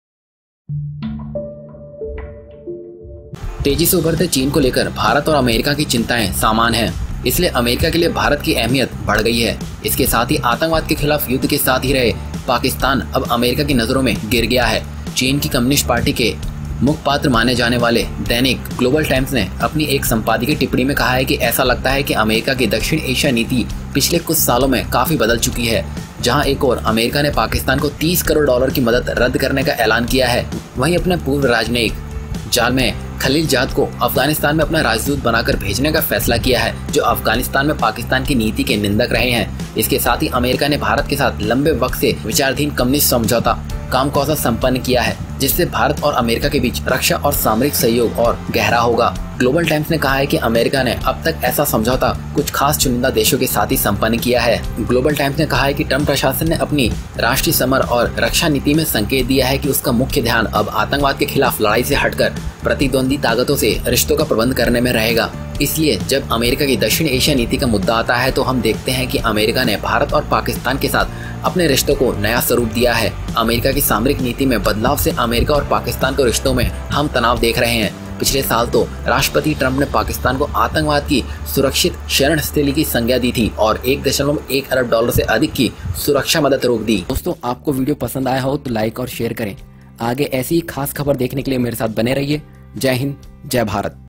दुनिया में India की अहमियत बढ़ने लगी अमेरिका ने लगा दिया भारत पर दांव China के छूटने लगे पसीने \
Daily Global Times, considered to be the mouthpiece of the Communist Party of China, said in an editorial comment that it seems that America's South Asia policy has changed significantly in the last few years
Zalmay Khalilzad

Sound Source